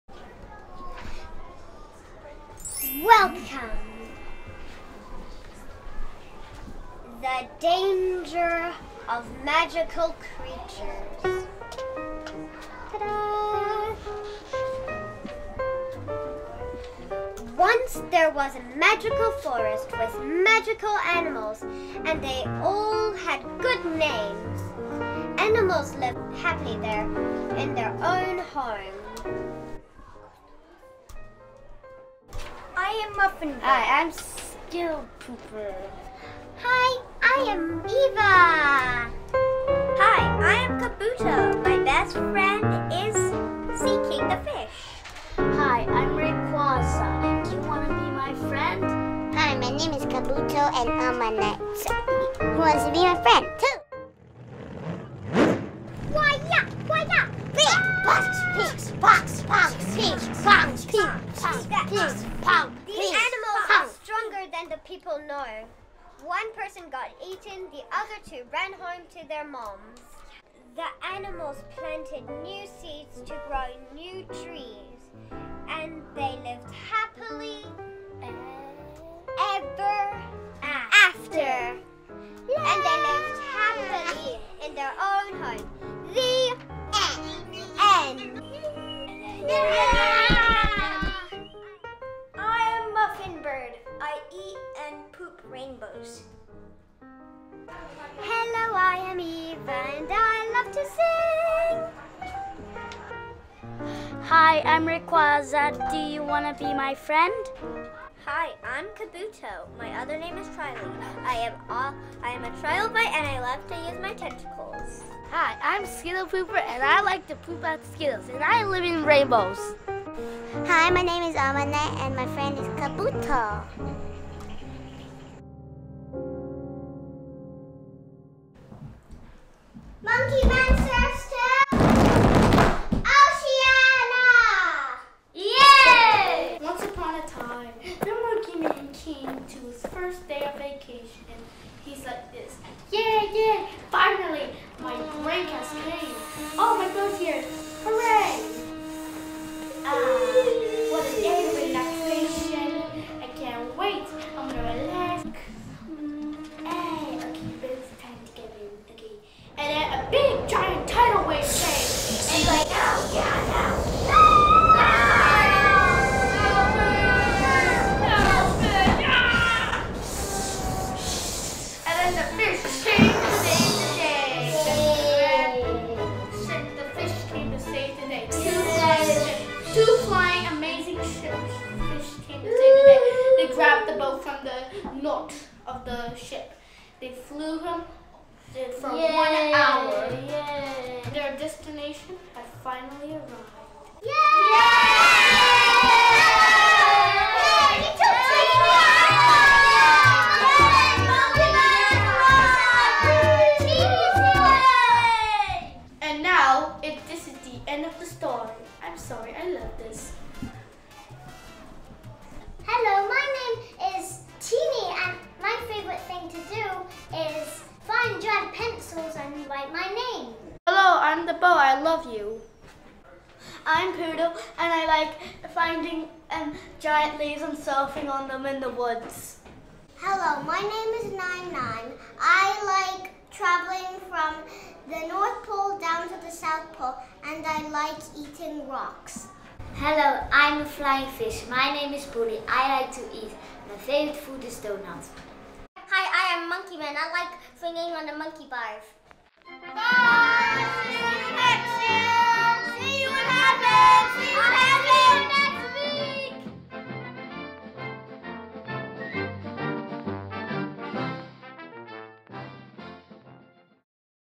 Nature Inspired Silhouette Film
The first film: The Danger of Magical Creature.
The second film: Monkeyman surfs to Oceana.

Two motion picture short films were created by primary school students from The Harbour School (Hong Kong), during the Art Interim week project, from a workshop called "Nature Inspired Silhouette Film" (Tutor: Damon and Rahel Lam © Mar 2017)